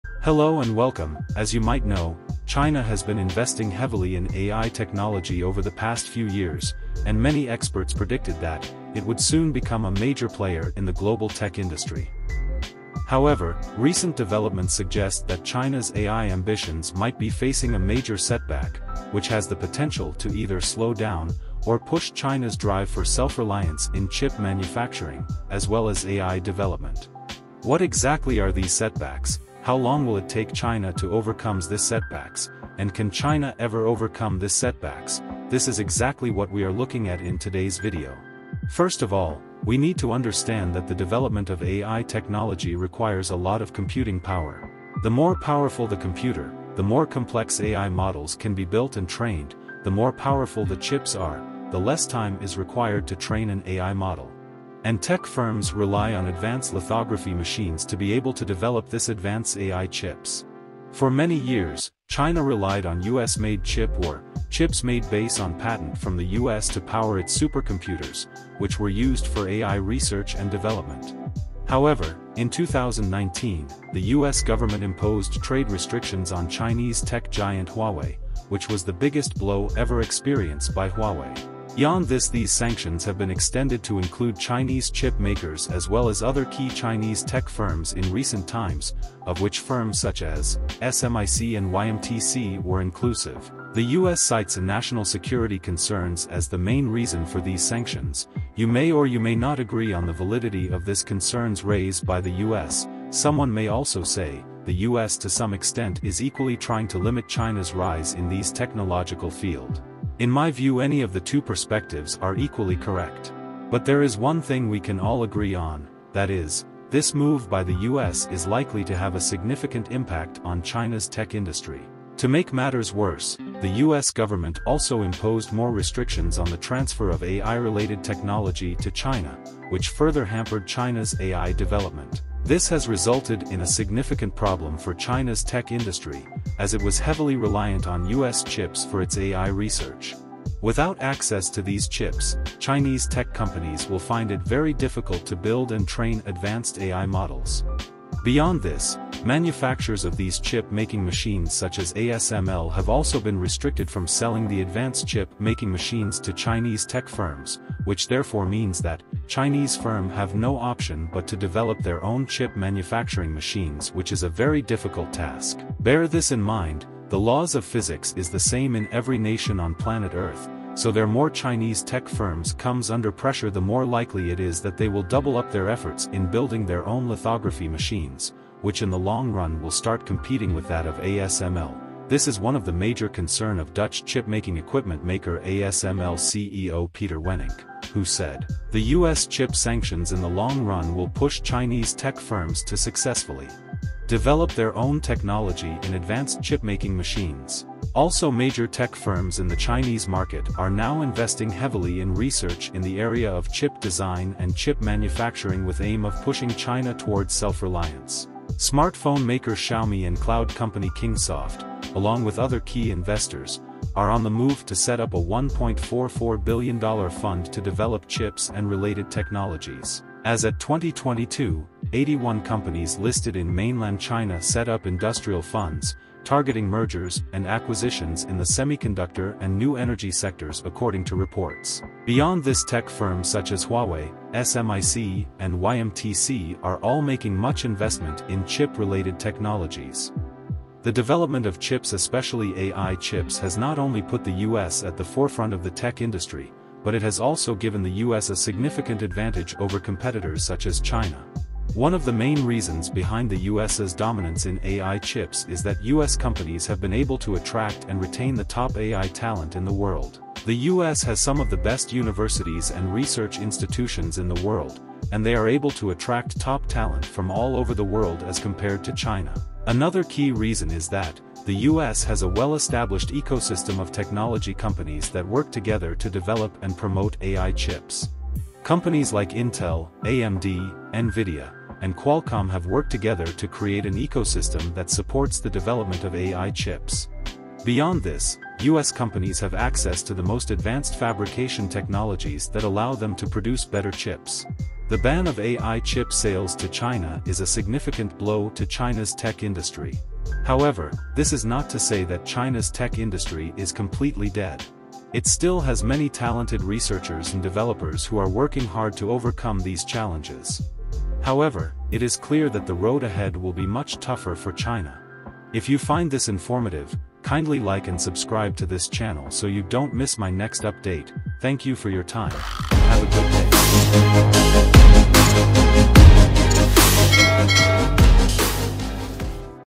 China's Ai Chip industry is in Resurrection mode
Hello and welcome, As you might know, China has been investing heavily in AI technology over the past few years, and many experts predicted that, it would soon become a major player in the global tech industry. However, recent developments suggest that China's AI ambitions might be facing a major setback, Which has the potential to either slow down, or push China’s drive for self-reliance in chip manufacturing, as well as AI development. What exactly are these setbacks, how long will it take China to overcomes this setbacks, and can China ever overcome this setbacks, this  is exactly what we are looking at in today’s video